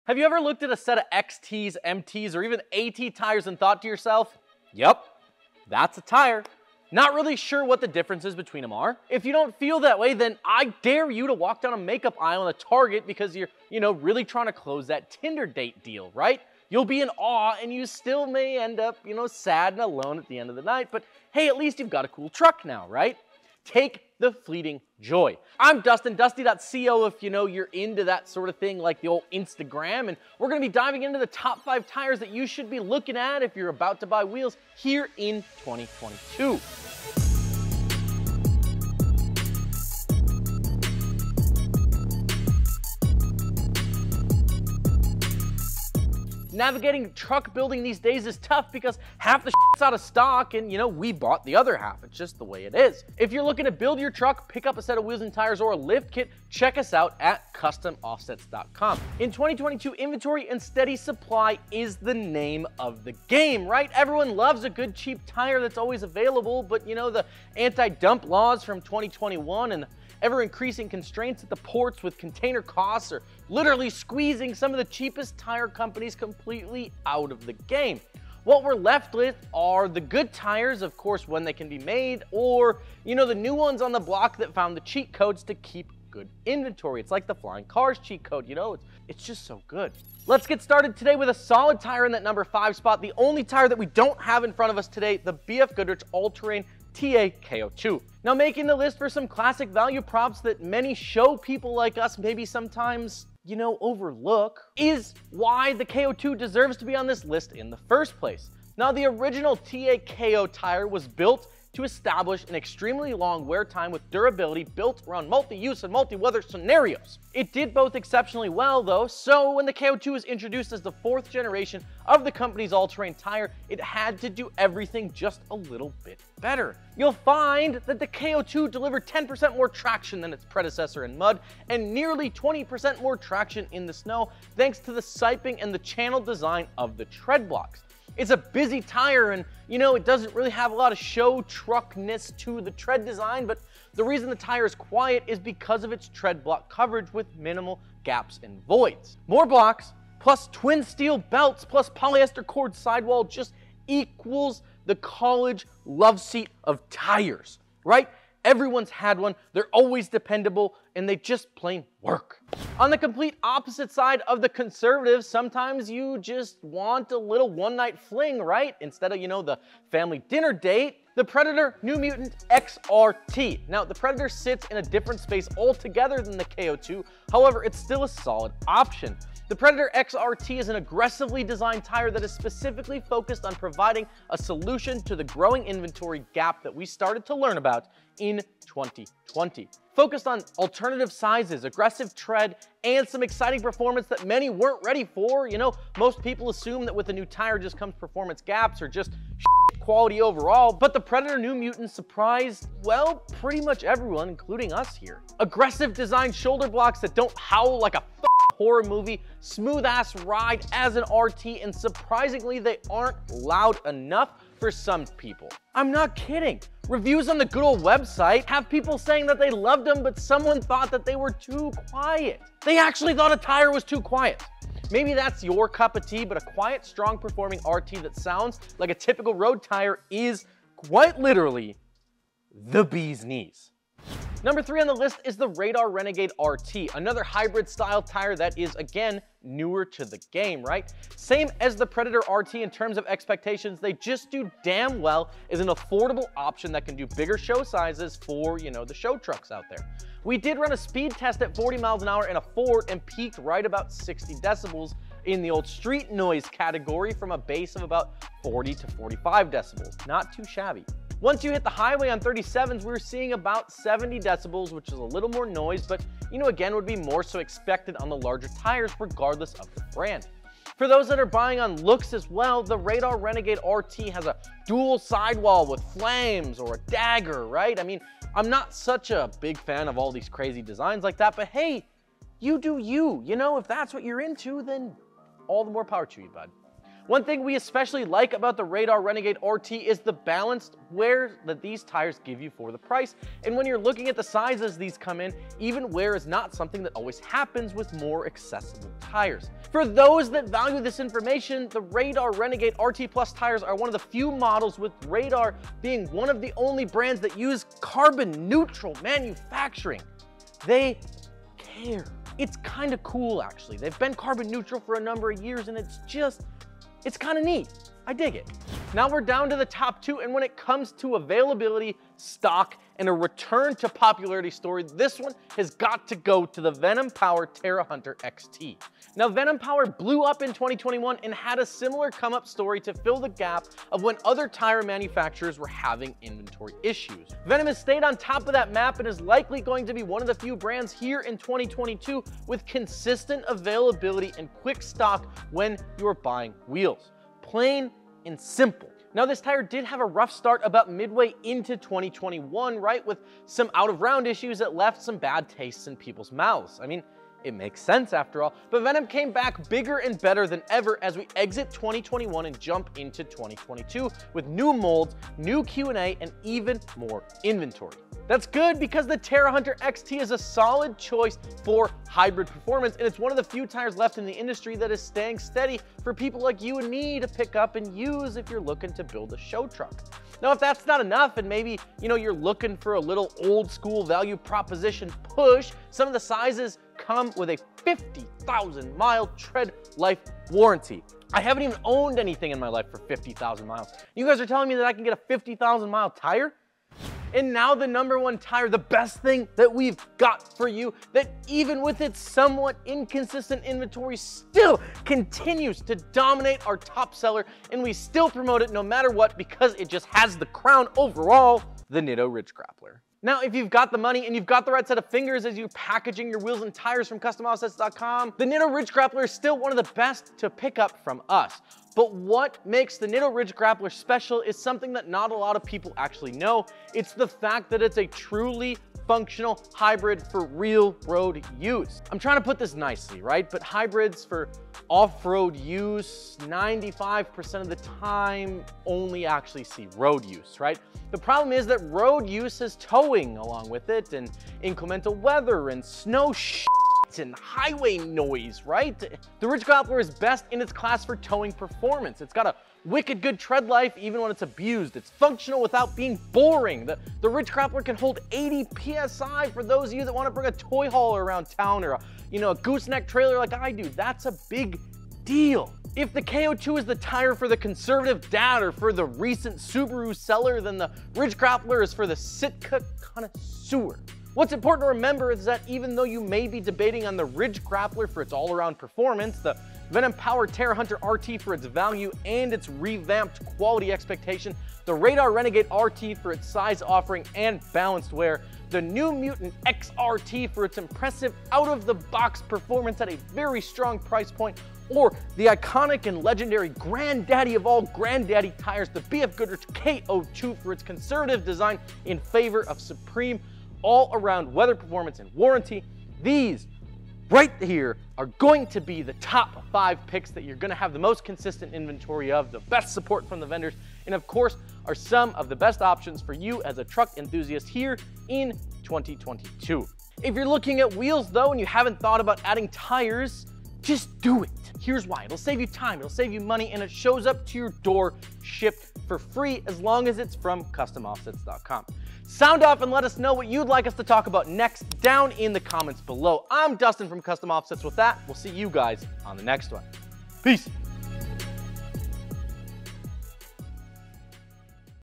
Top 5 Tires of 2022!
Take our word for it and grab a set of the top tires in the market today

In the market for some tires for YOUR truck? Don't know what to pick for your build? What the heck is a tire?! Today, Dustin reviews the top 5 tires of this year!

0:00 Intro
1:30 BFG KO2
2:42 Predator New Mutant XRT
3:54 Radar Renegade R/T
5:18 Venom Power Terra Hunter X/T
6:36 Nitto Ridge Grappler
7:54 Outro